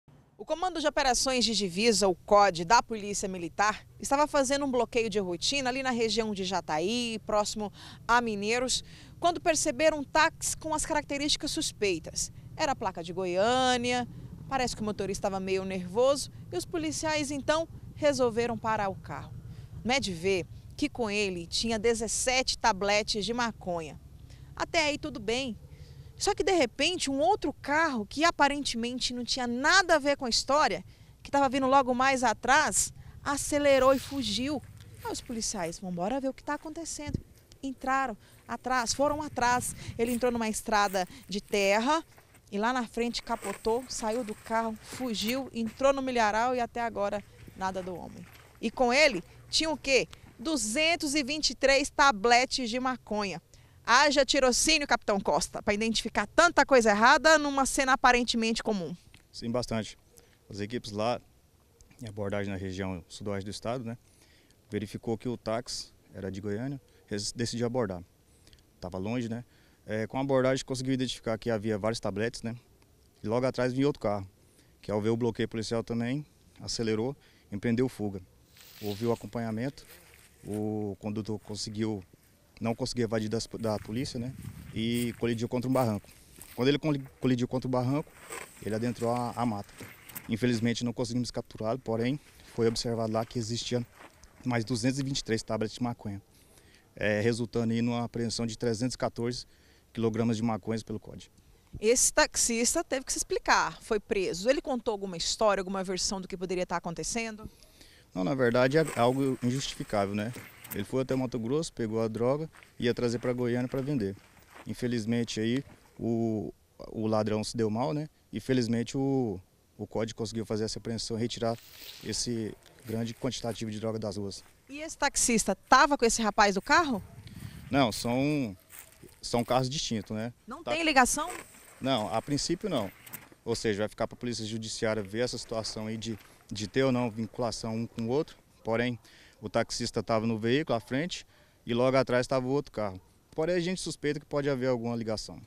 314 KG DE MACONHA APREENDIDOS EM DOIS CARROS
314 quilos maconha foram apreendidos em dois carros, entre Jataí e Mineiros. Um dos motoristas é um taxista que foi buscar parte da droga no estado do Mato Grosso, para vender em Goiânia.